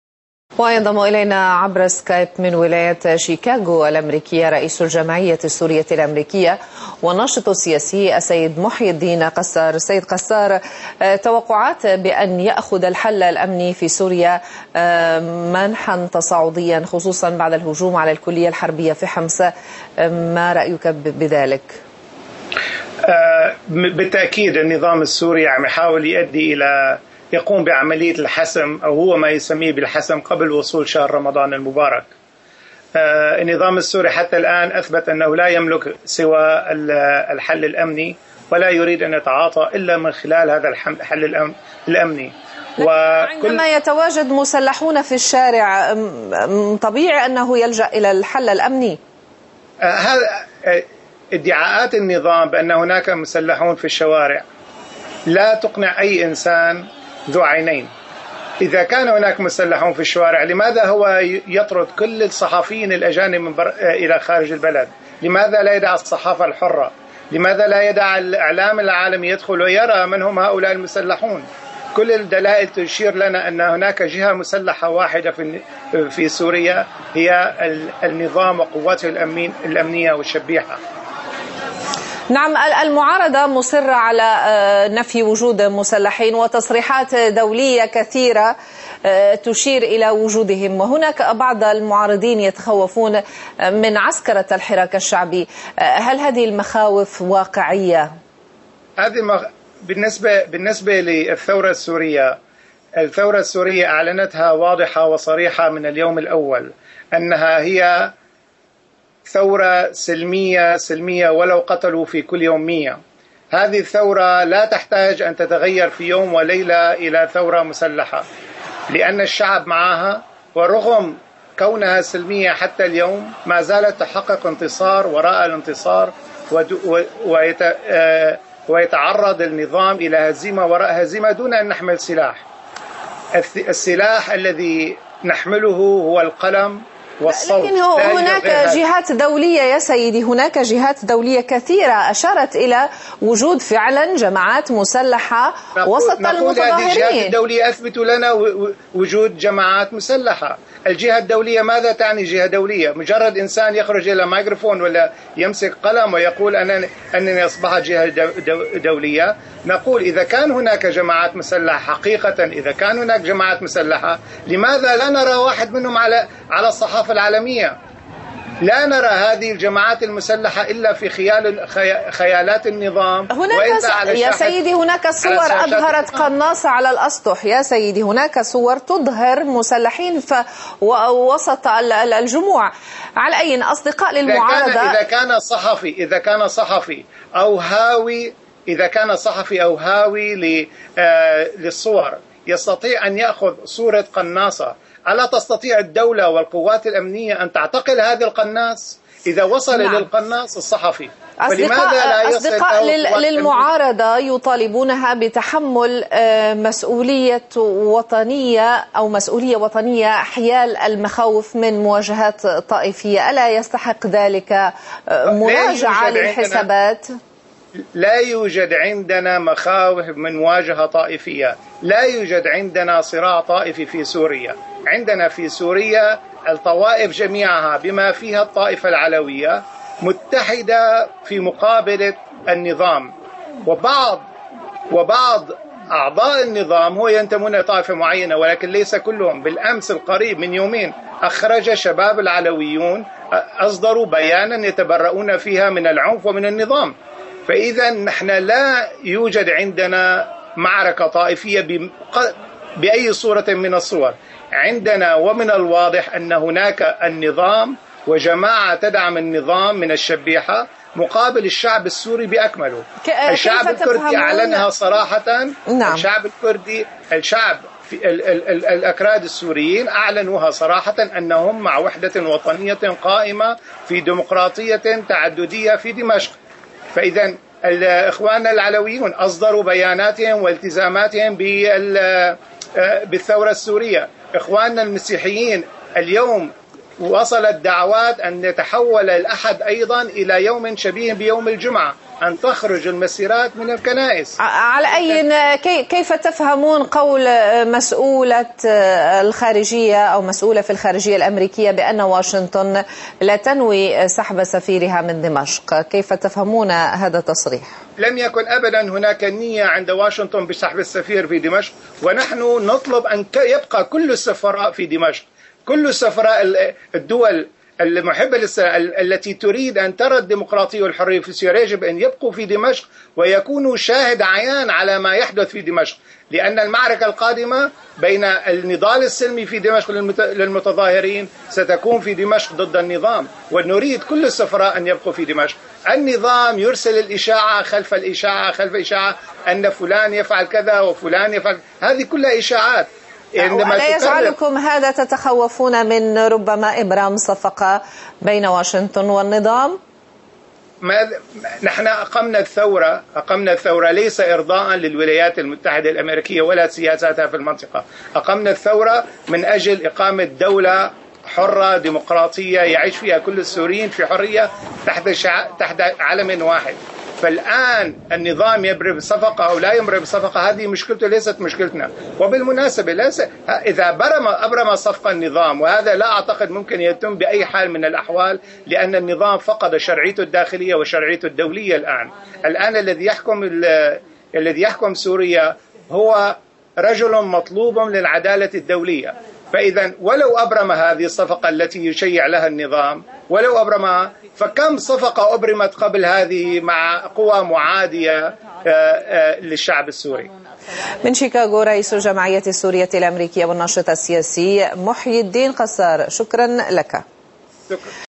روسيا اليوم: حول الثورة السورية وسلمية الثورة 24 تموز 2011
اشار رئيس الجمعية السورية الأمريكية والناشط السياسي محيي الدين قصار في حديث مع "روسيا اليوم" من شيكاغو الى النظام السوري "يريد حسم الامر قبل حلول شهر رمضان المبارك، وقد اثبت انه لايملك سوى الحل الامني وانه لايريد التعاطي الا عبره". وقال ان وجود مسلحين في الشارع هو "ادعاء من قبل النظام، فاذا كان ذلك صحيحا لماذا لايسمح لوسائل الاعلام بالدخول؟".